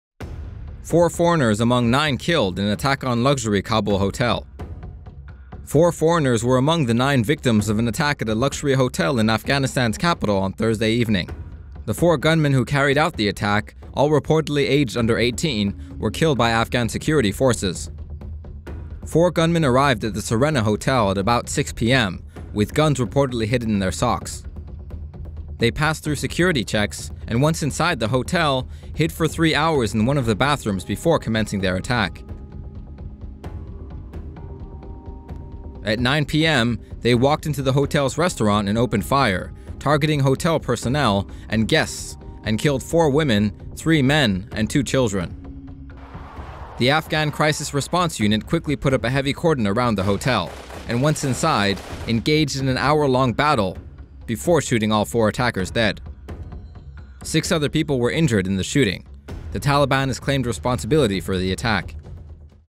Taliban gunmen hide guns in socks before attacking luxury Kabul hotel
Four foreigners were among the nine victims of an attack at a luxury hotel in Afghanistan's capital on Thursday evening (March 20), according to a BBC report.

The Afghan government, has blamed the Kabul attack on Pakistan's ceasefire with the Taliban, saying it has enabled the militants to focus their attention on Afghanistan.

The four gunmen who carried out the attack, all reportedly aged under 18, were killed by Afghan security forces.

The militants arrived at the Serena hotel at around 6 p.m. with guns hidden in their socks. They went through security checks claiming to be diners at the hotel's special buffet, according to an AFP report.

Once inside the hotel, they hid in one of the bathrooms for around three hours. At 9 p.m. they entered the hotel's restaurant and opened fire on guests and hotel personnel.

Outside the hotel, the Afghan Crisis Response Unit quickly put up a heavy cordon and once inside engaged in a more than an hour-long battle which ended with in the deaths of all four attackers.

Terrified guests fled the restaurant, hid in their rooms or found shelter in safety bunkers around the complex, the New York Times reported.

Jane Ferguson, an Al Jazeera television journalist staying at the hotel, said on Twitter that she "just got out of Serena hotel safe. Horrible few hours with furniture against my door."

The foreign nationals killed were from Canada, India, New Zealand and Pakistan, according to Reuters. Four women, three men and two children were killed in the attack.

Six others were wounded in the shootings, Deputy Interior Minister General Mohammad Ayub Salangi was quoted by the BBC as saying. An Afghan MP, Habib Afghan, was shot in the face, stomach and leg and is now recovering in a hospital.
The Taliban has since claimed responsibility for the attack.

The hotel lies in the centre of Kabul, less than 1 kilometer from the presidential palace.

A popular destination for wealthy visitors in Kabul, it has been one of the most frequent targets of the Taliban, and it has been successfully attacked several times before, the BBC reported.